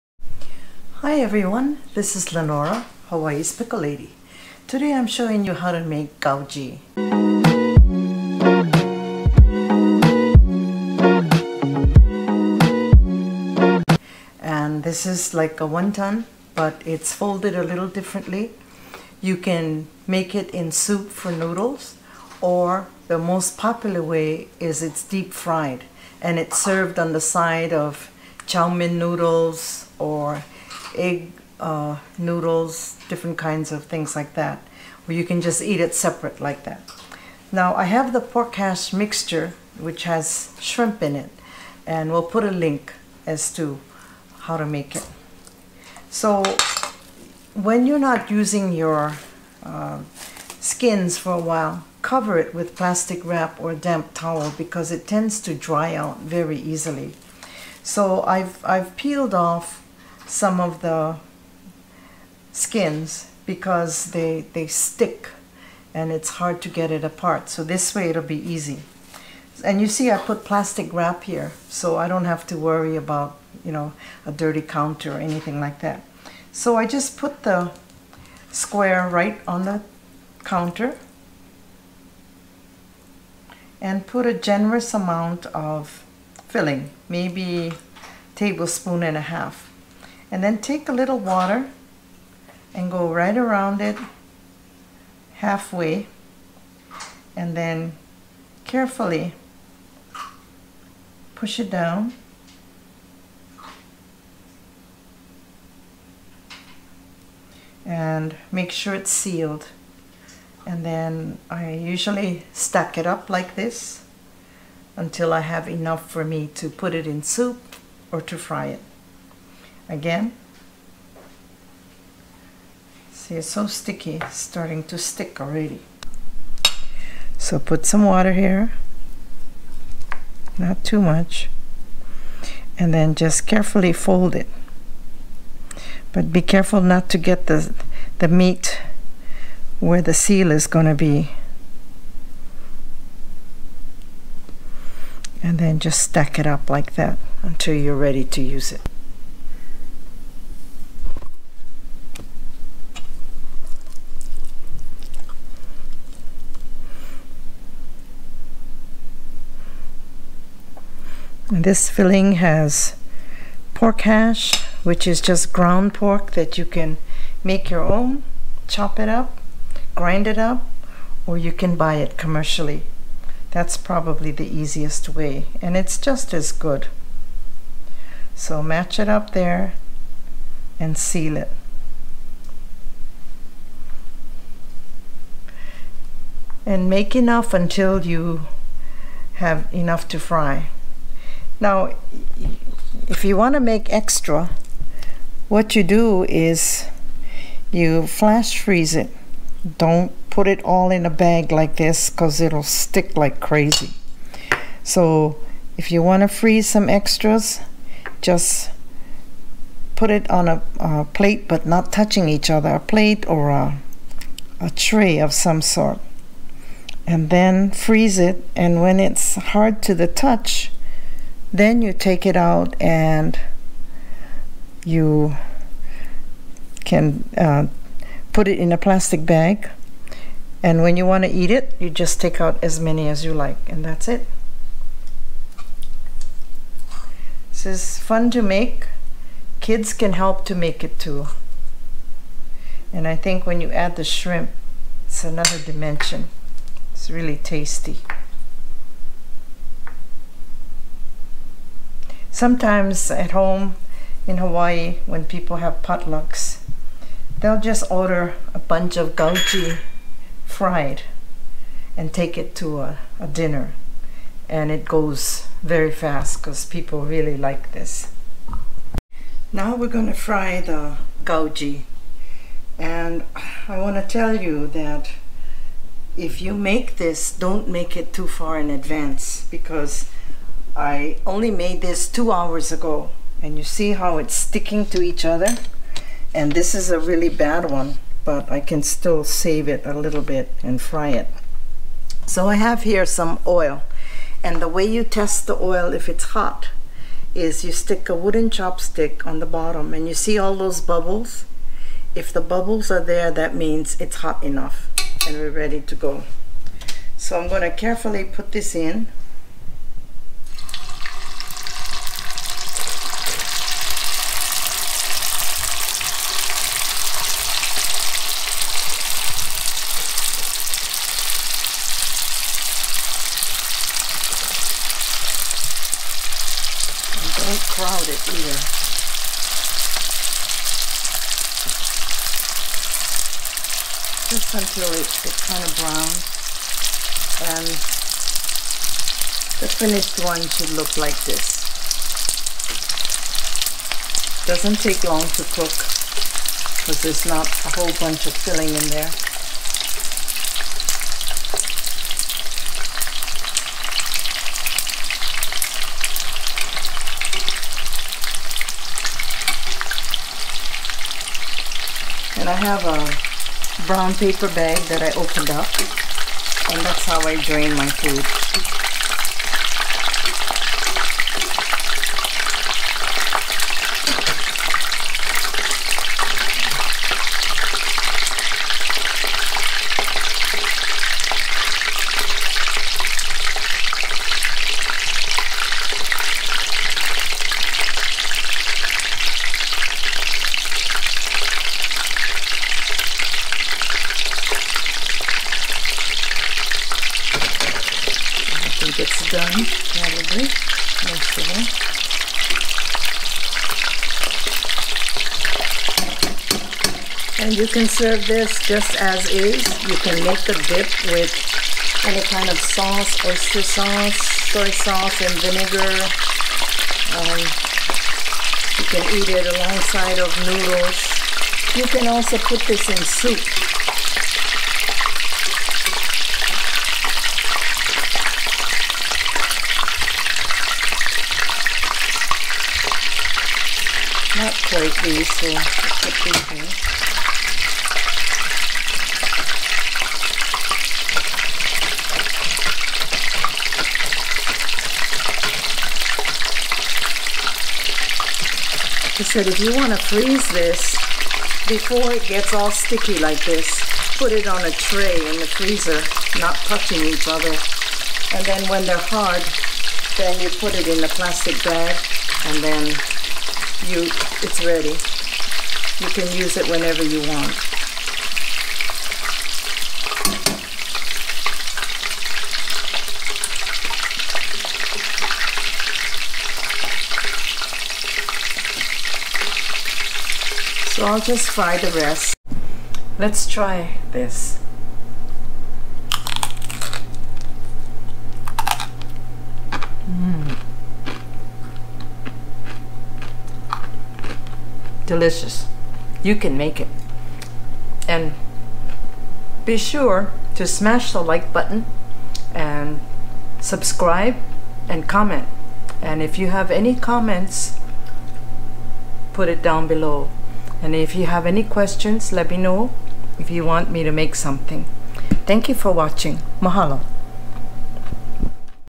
How To Make Crispy Gau Gee 😁 | Easy to make
Crispy gau gee is a glorified won ton with more filling, folded in half and deep fried.  It is so crunchy and delicious and can be dipped in a variety of sauces such as:  soy sauce, soy paste, vinegar soy mix, oyster sauce, kochujang (Korean  hot chili bean paste) or any other sauces with sesame oil, green onions, etc.  It usually accompanies various Chinese fried noodle dishes or eaten as a snack or side dish.

If you're in Oahu, Hawaii or Las Vegas, Nevada be sure to check out my PICKLE PASSION COOKBOOK in the library!

You can find this recipe under Pork.

If anyone has made this please post in Facebook.